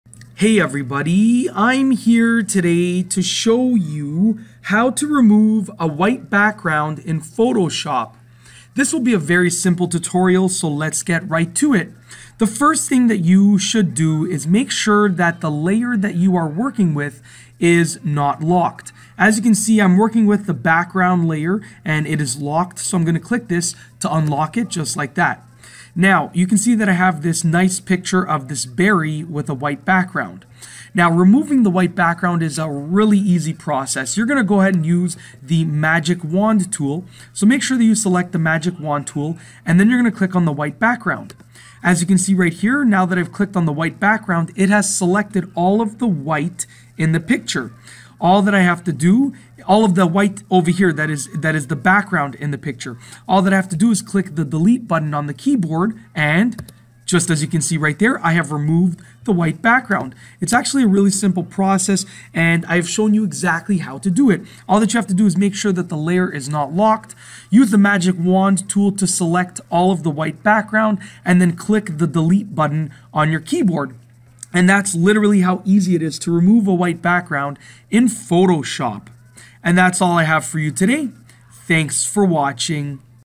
How To Remove A White Background In Photoshop-Tutorial
In this video, I show you how to remove a white background in Photoshop. It is really easy to do, and you can do it, even if you are a complete beginner to Photoshop! I used a simple picture of a berry to remove the white background from, but you can use this method to select the background of a busy picture as well! Photoshop is really easy to use (the basic functions), and I made this tutorial to show you how to do something that many people want to learn how to do!